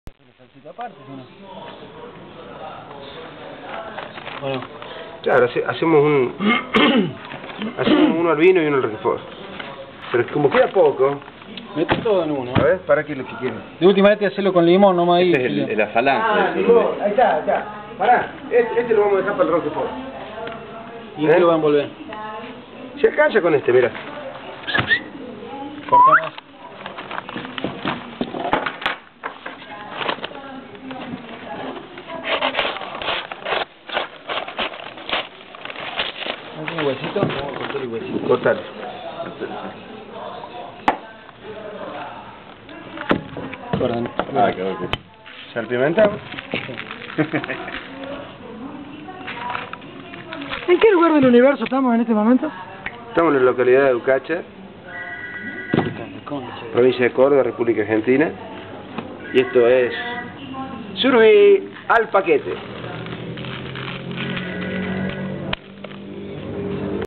SURUBÍ AL PAQUETE EN UCACHA - 3º parte
El Negro y el Franco cocinan un surubí.